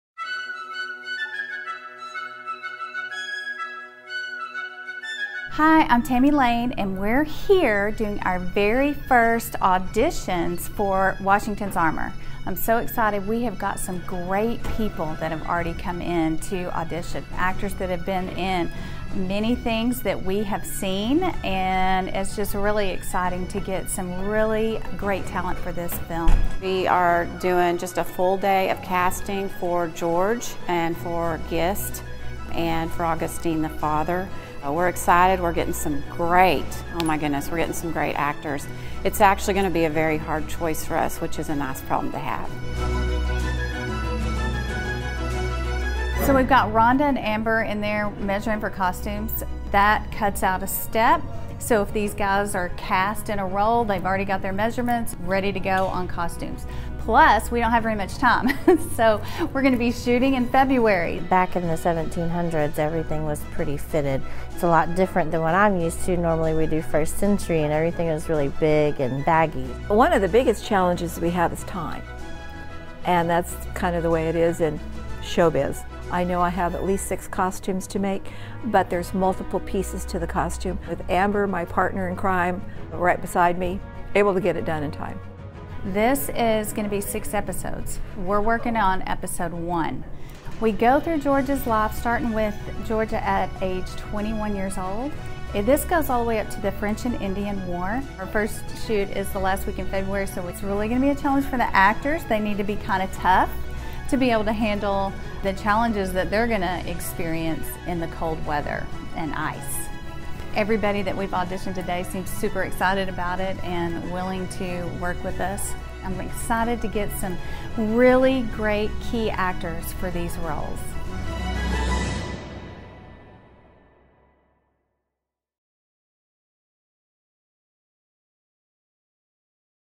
CASTING a new show!
CASTING has begun on a brand new show! Who will play young George Washington fighting to survive the treacherous wilderness? Who will play Christopher Gist, Washington's trusted side-kick? Coming Soon!